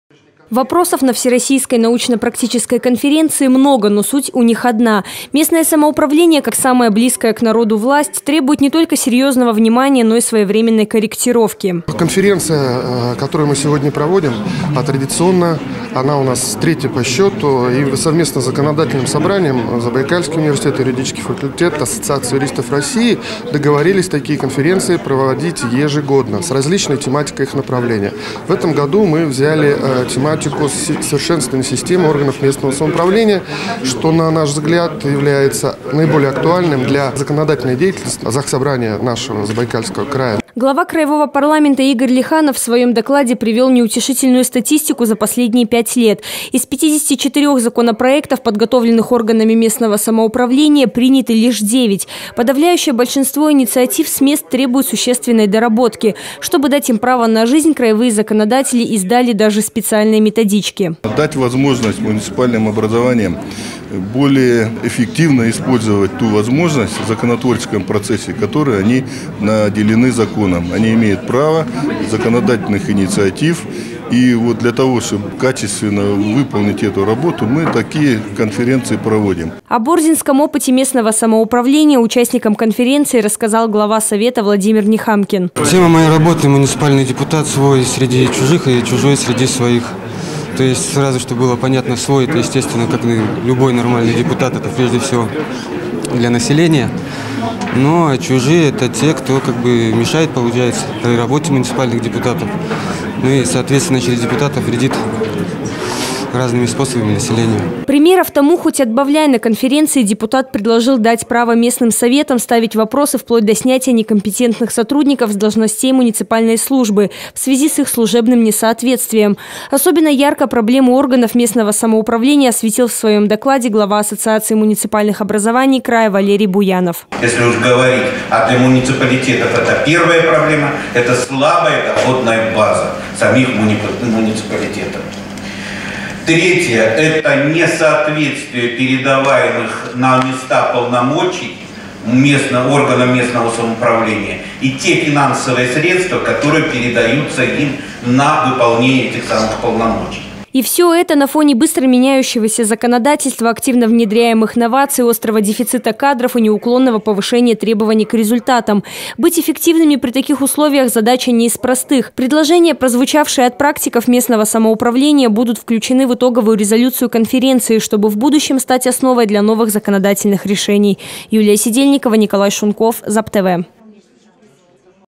Законопроекты, подготовленные органами местного самоуправления, требуют серьёзных доработок
Вопросов на Всероссийской научно – практической конференции много, но суть у них одна. Местное самоуправление, как самая близкая к народу власть, требует не только серьезного внимания, но и своевременной корректировки.